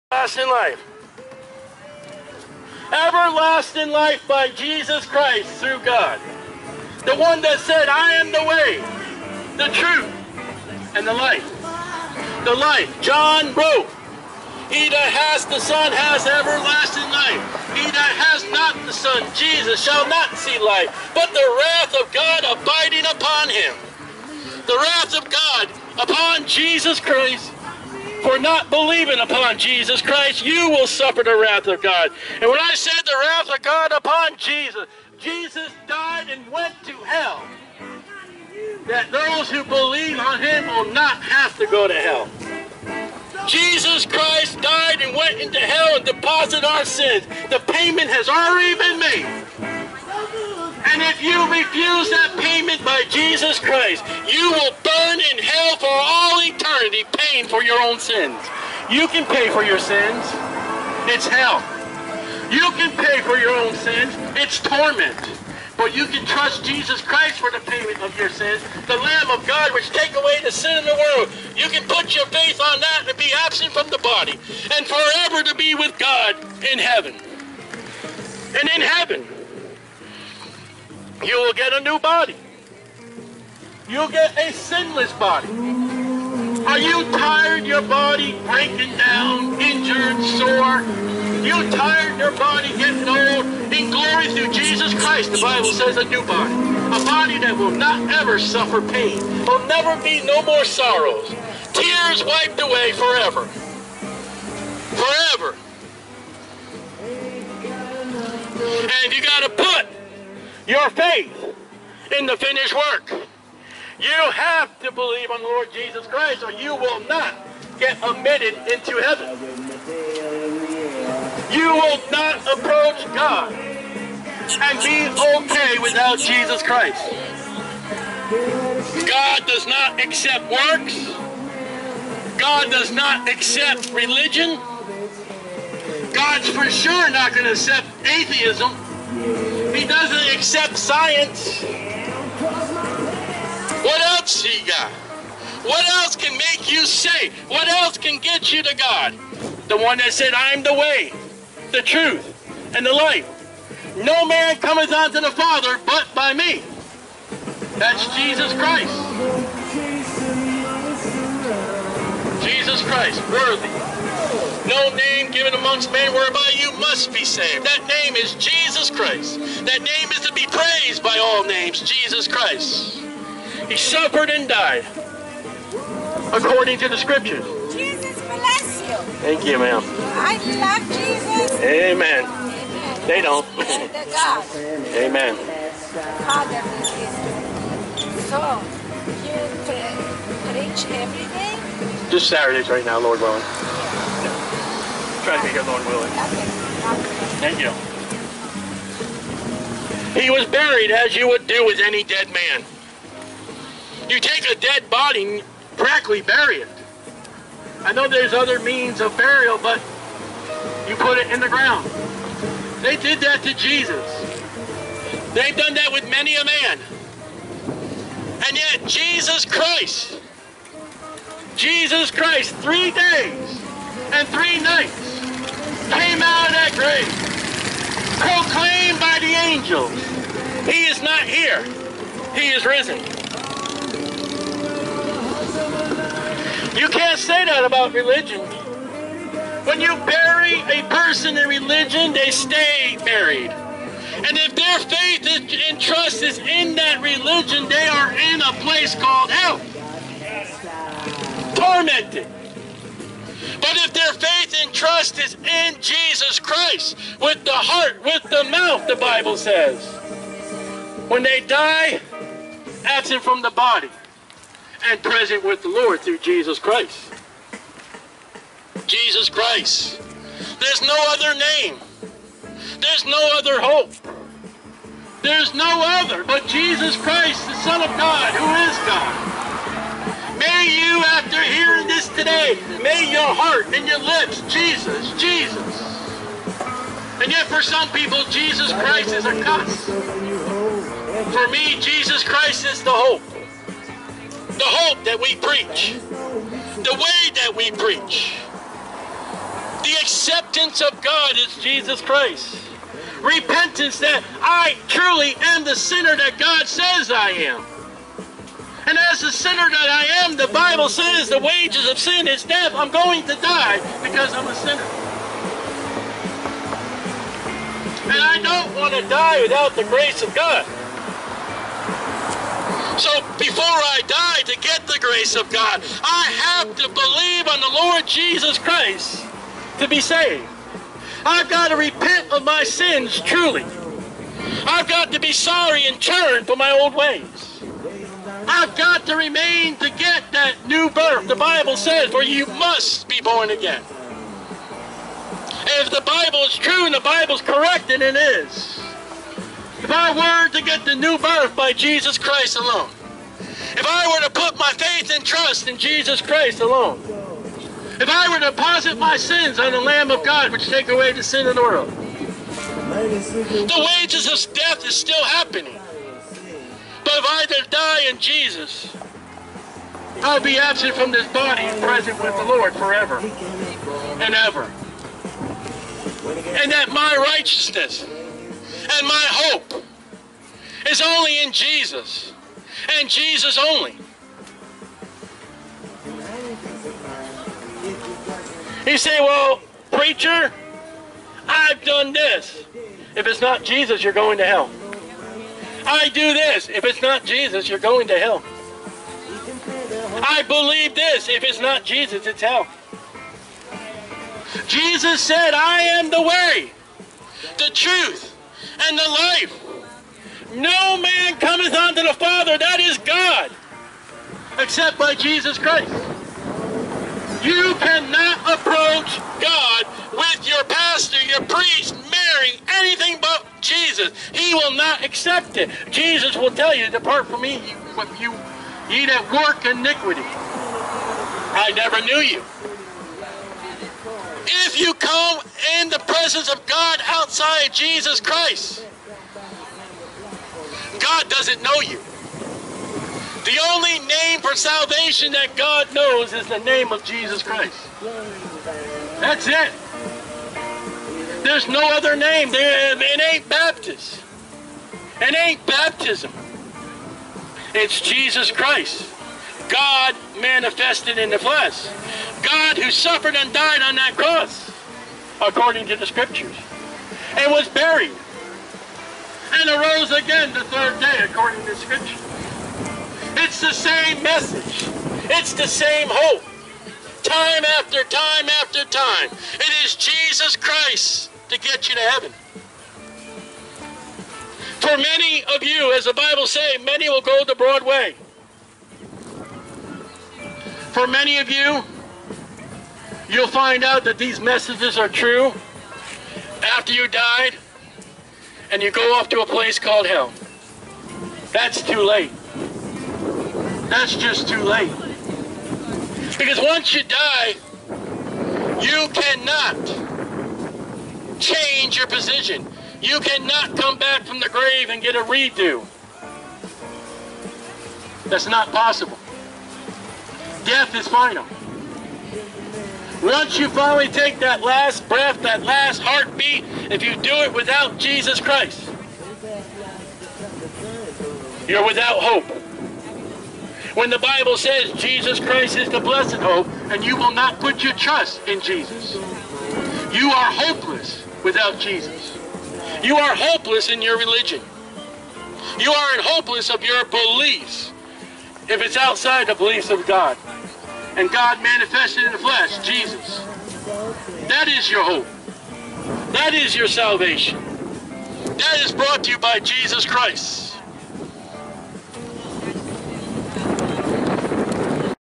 CISP The Same Gospel Still Preached! 2 The Right Side
Although there is a DJ involved, Jesus Christ still stands! The blood of Jesus cleanses us from all sins by dying on the cross, being buried in a tomb, and rising again on the third day. All you have to do is believe on the LORD JESUS CHRIST and thou shalt be saved; there is no other way but Christ's.

Write To Us:
Jesus Christ
The Blessed Hope